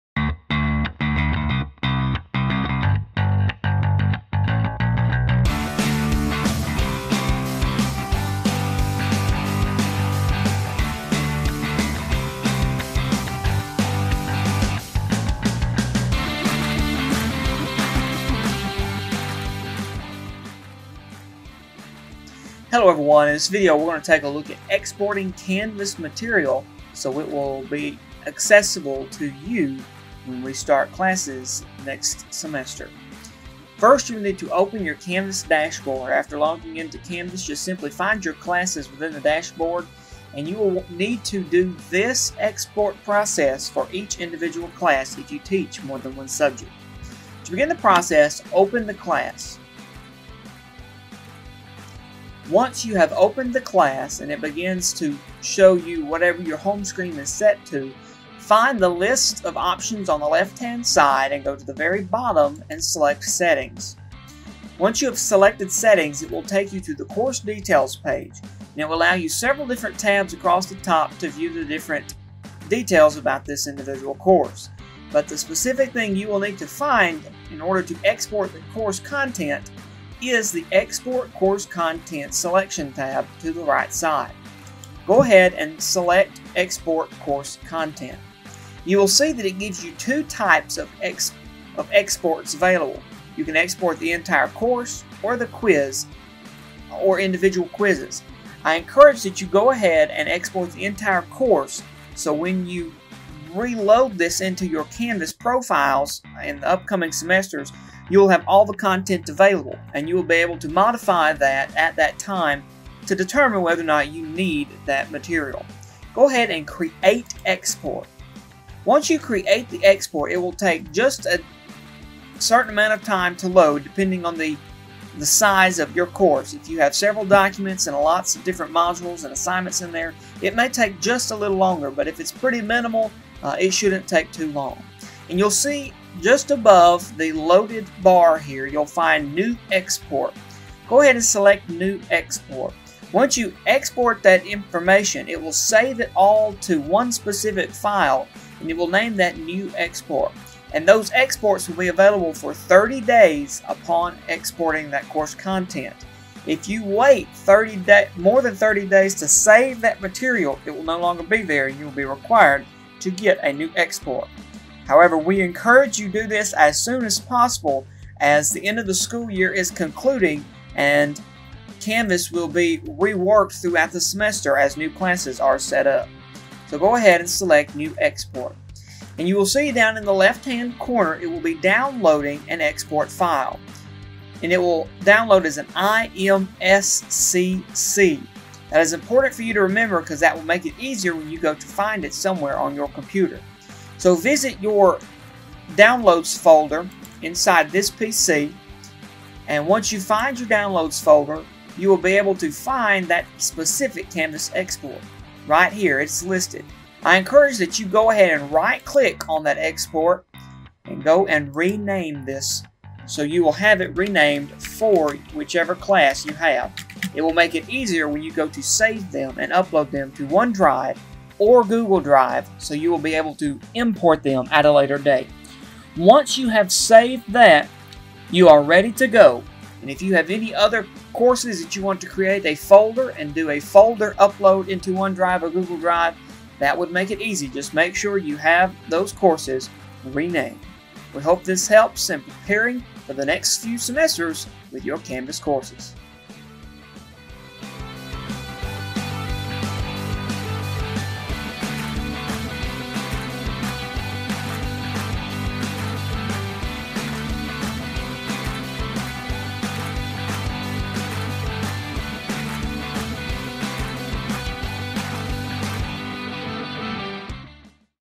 Exporting Canvas Course Content
This video details the process of exporting canvas course to a desktop machine for uploading to OneDrive or Google Drive.